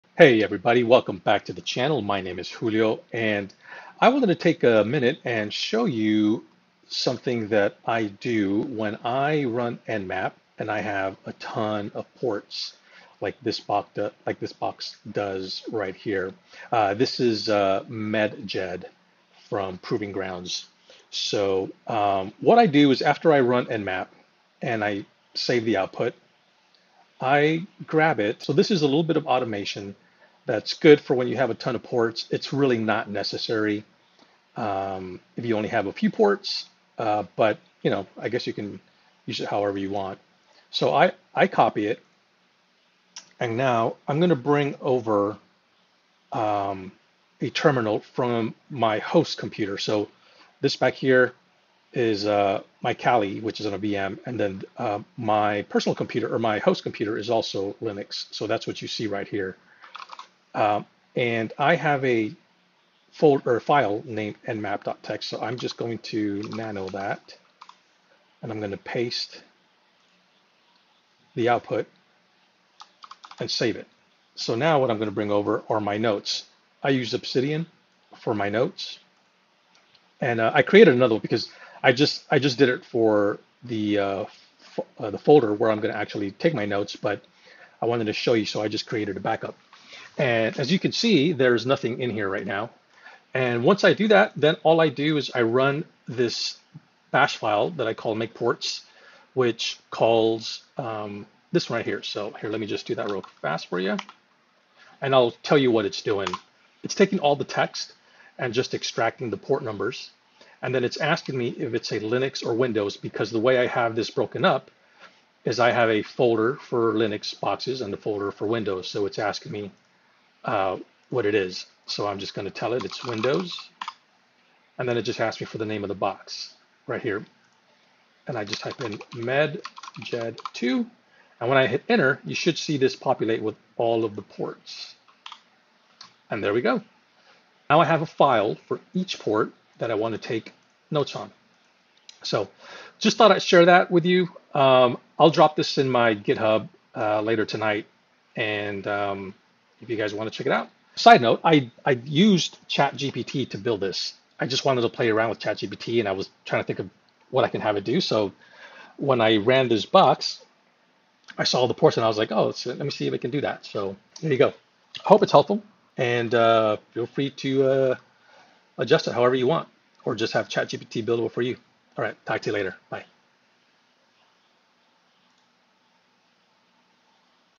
I used ChatGPT to create files from Nmap in Obsidian!
I had ChatGPT help me build a script that creates an Obsidian note for each port returned from an Nmap scan.  The scripts are designed for my specific use case and build the file path to where I store my Obsidian .md files but you can adjust them to build the path that matches your needs.